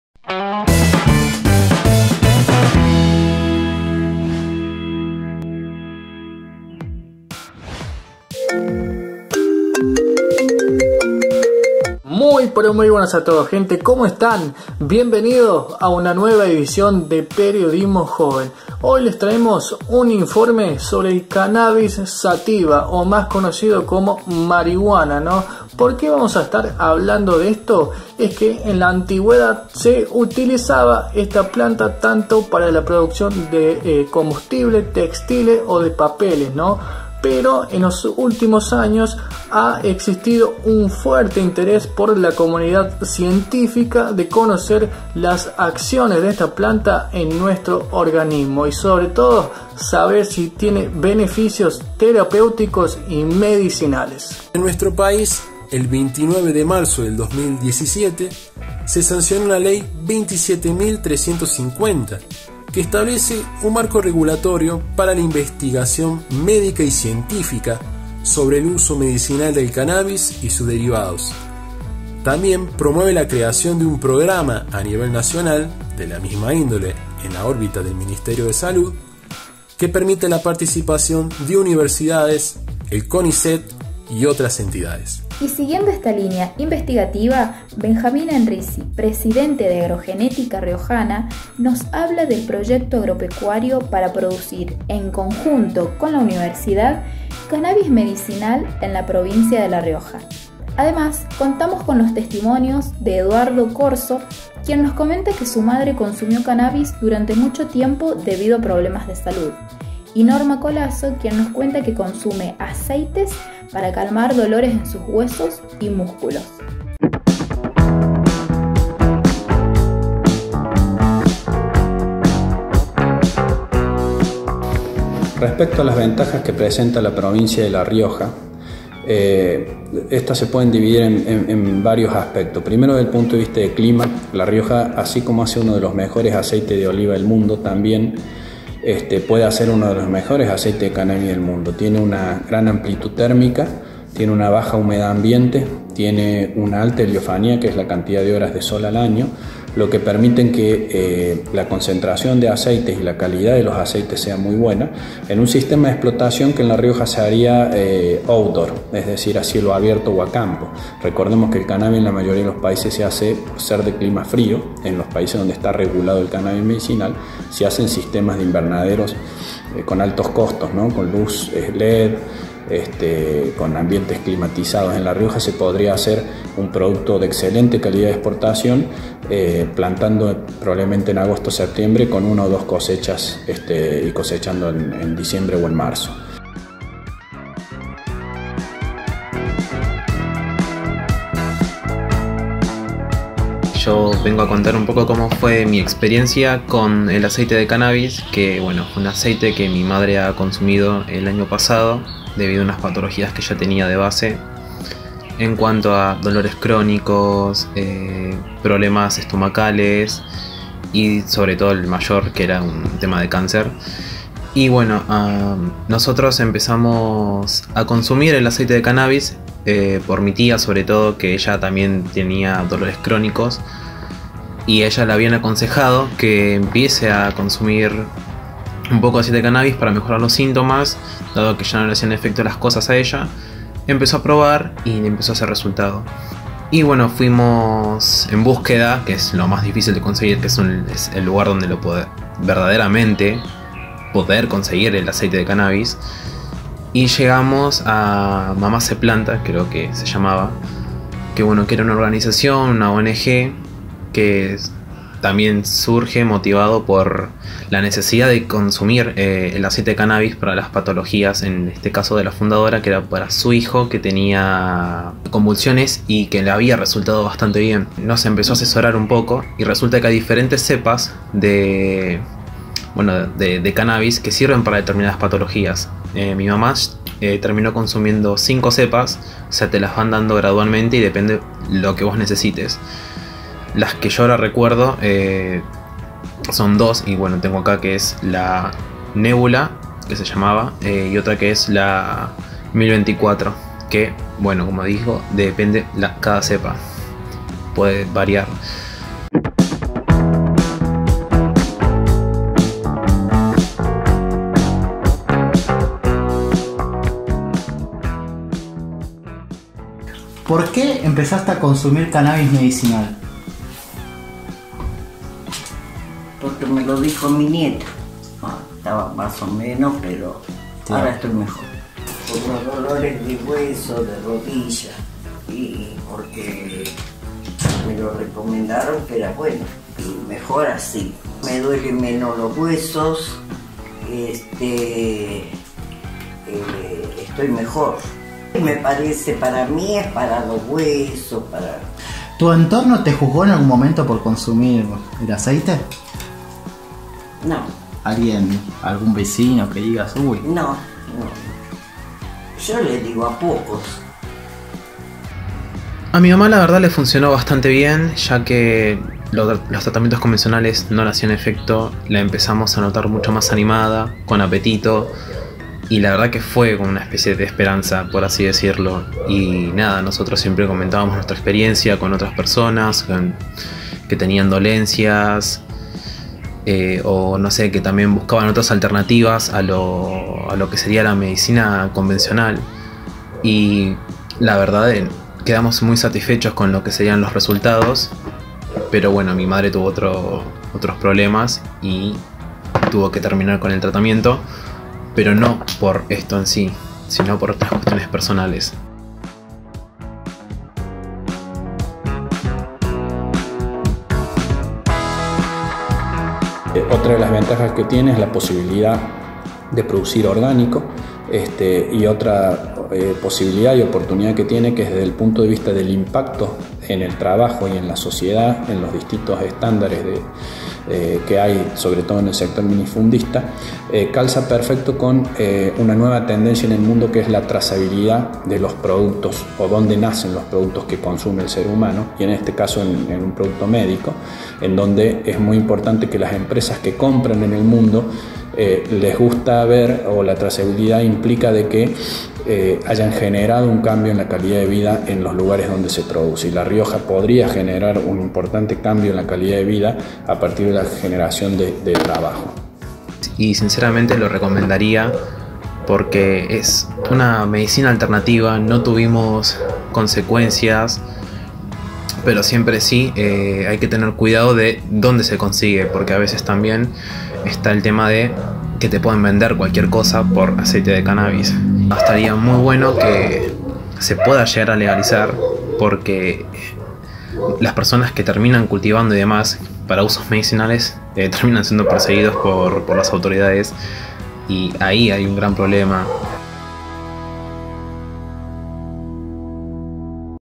Cannabis medicinal: su desarrollo en La Rioja
Cuáles son las ventajas climáticas para las plantaciones en la provincia y los beneficios del consumo para enfermedades, son algunos interrogantes planteados en este informe, producido por estudiantes de Licenciatura en Comunicación Social.